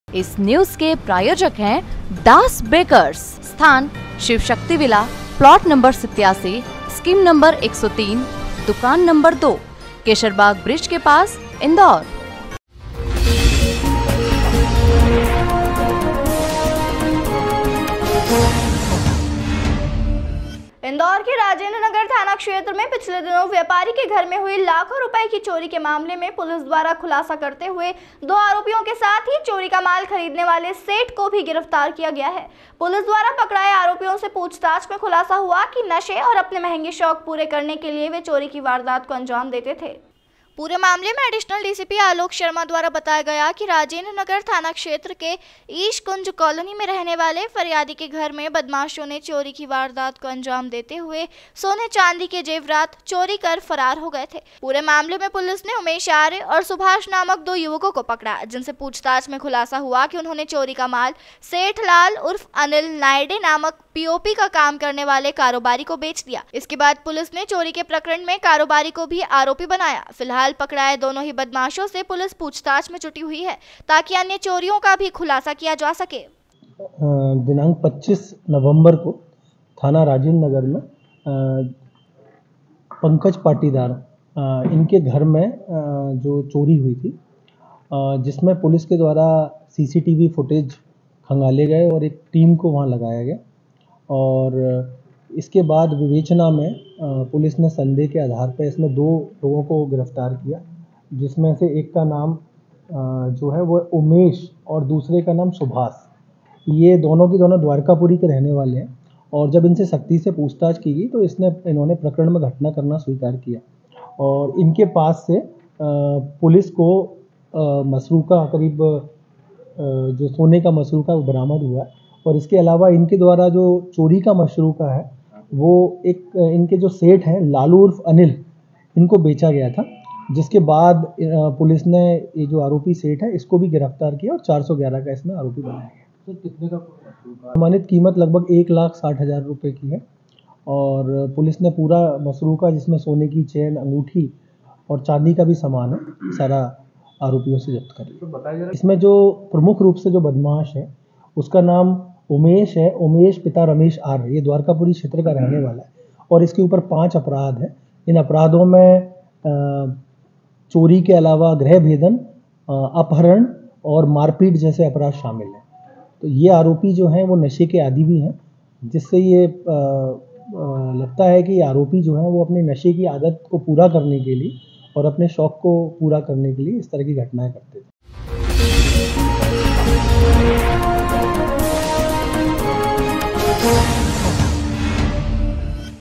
इंदौर में नशे व महंगे शौक पूरे करने के लिए की थी चोरी
इंदौर में नशे व महंगे शौक पूरे करने के लिए की थी चोरीrashtrasamachar